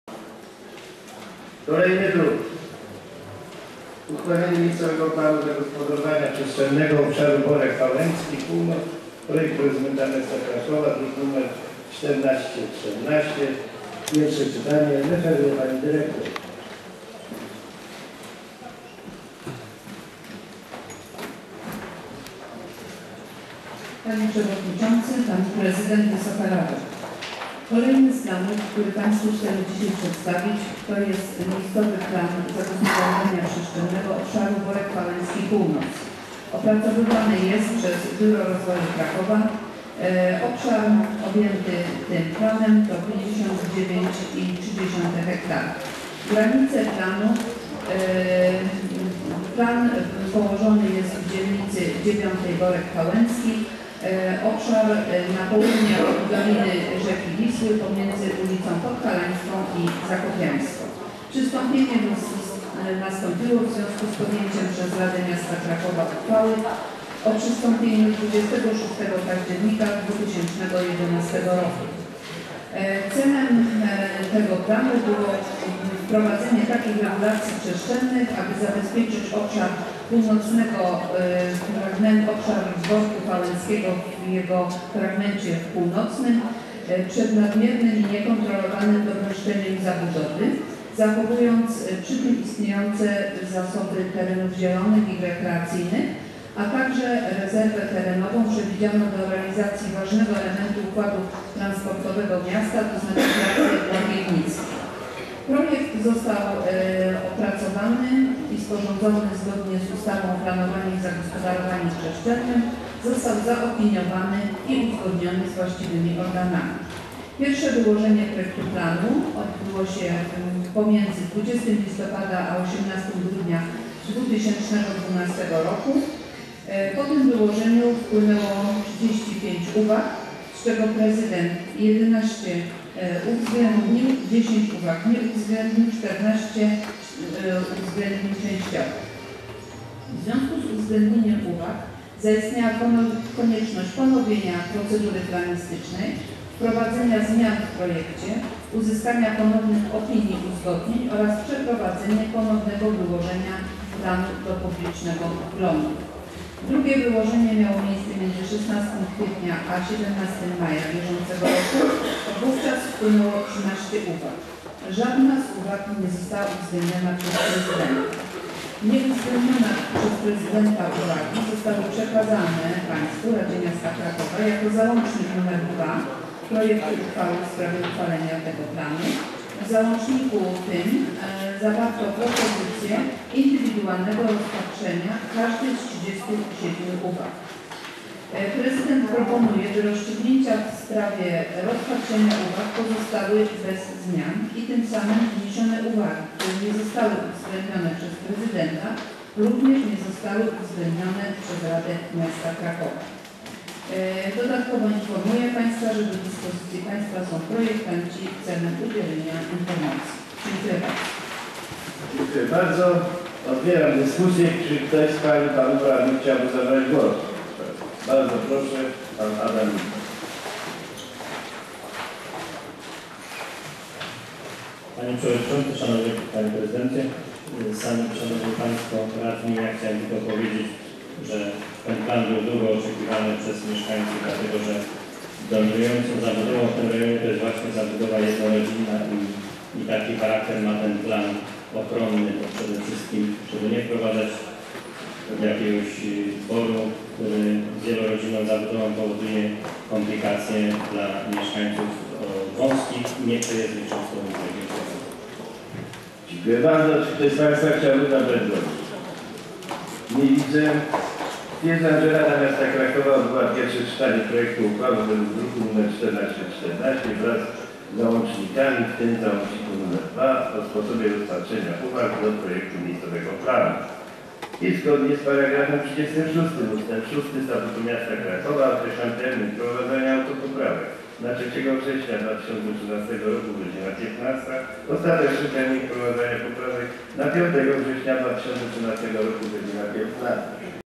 6. MPZP obszaru "Borek Fałęcki -Północ"
Uchwalenie miejscowego planu zagospodarowania przestrzennego obszaru "Borek Fałęcki -Północ"
Projektodawca Prezydent Miasta Krakowa, referuje Elżbieta Koterba
Pozytywna opinia prawna Sebastian Pietrzyk